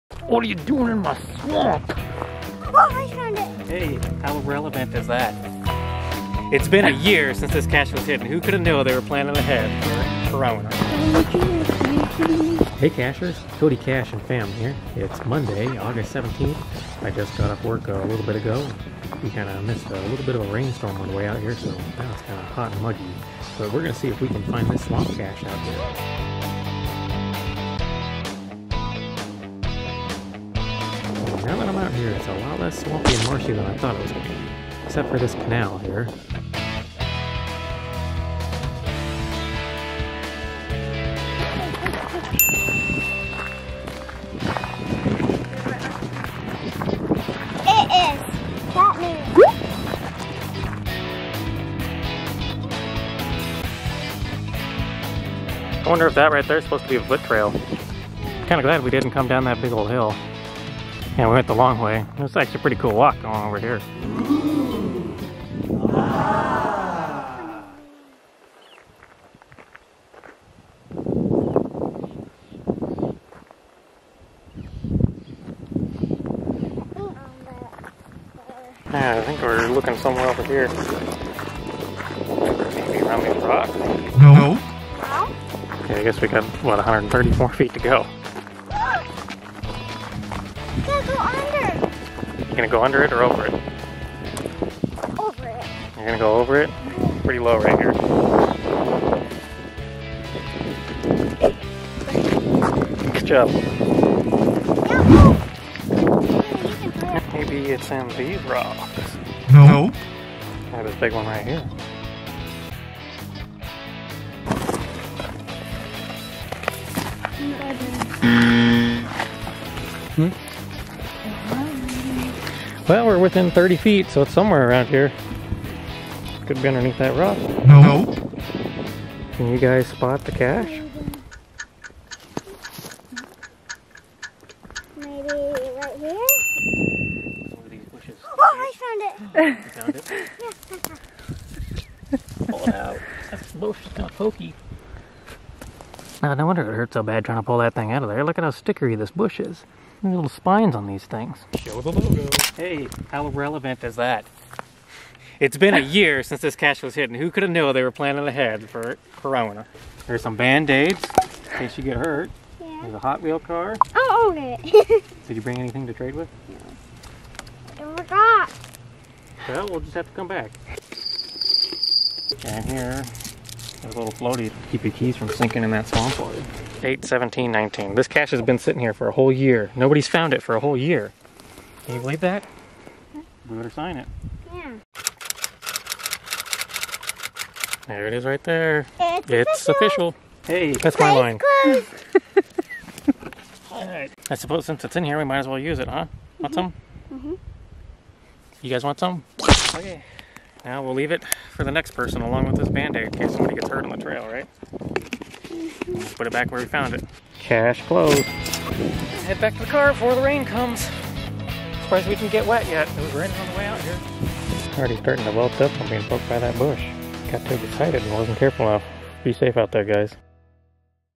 Swampy Canal Cache 🐸 [1 YEAR Since Last Find!]
Will this Geocacher and FAMily find the geocache hidden along the marshy, swampy canal? And did I mention it’s been a whole year TO THE DAY since this cache was last found? Guess you’ll have to watch to find out!

Weekday geocaching, filmed 8/17/2020.

In a hurry?
Need to rewatch your favorite part?
The suspense is too much—take me to the cache!

TIME STAMPS
0:12 Intro/About This Cache 🐸
0:42 Official Navigator 🧭
1:04 15 Second Panoramic View 🏞
1:19 The Search
2:09 Can YOU 👀 Spot the Cache?
Stay Excellent!
and
Keep on Cachin’ on.
Free Open Source Emojis courtesy of